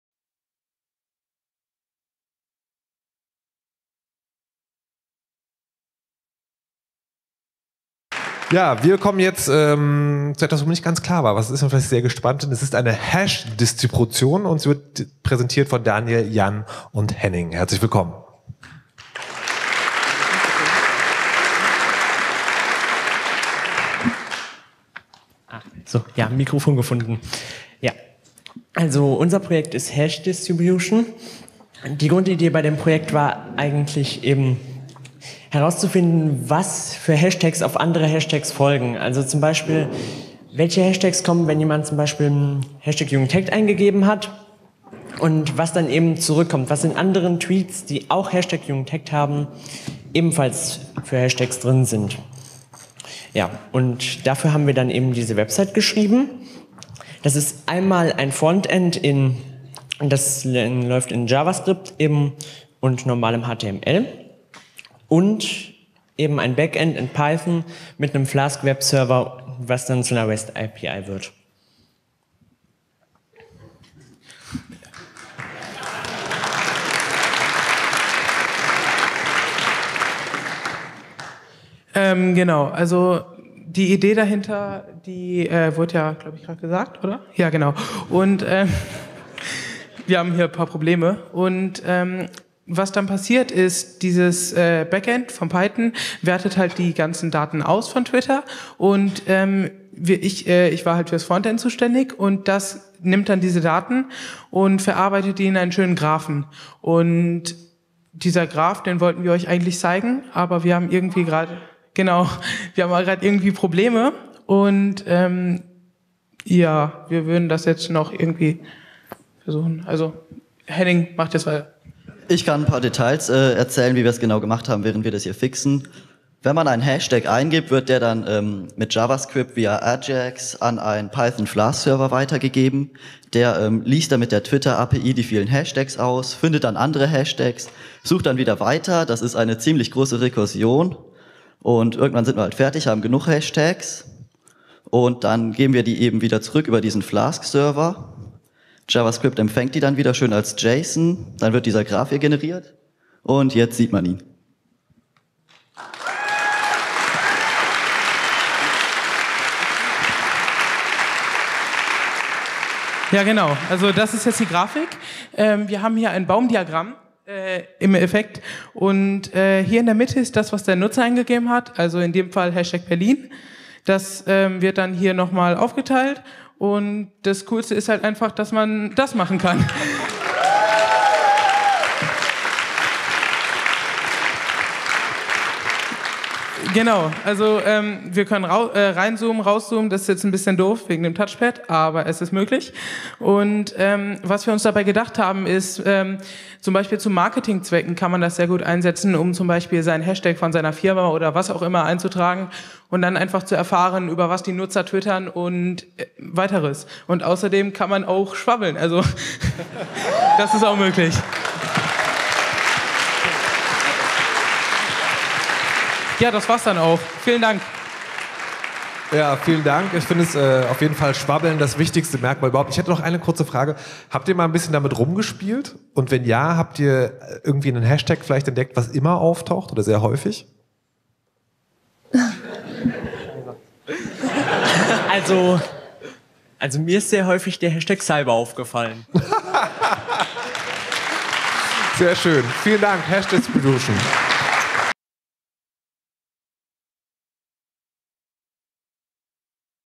Hashdistribution | Jugend hackt Berlin 2016
Hashdistribution ermittelt ähnliche Hashtags auf Twitter und visualisiert sie mit D3.JS. Von Daniel, Jan und Henning.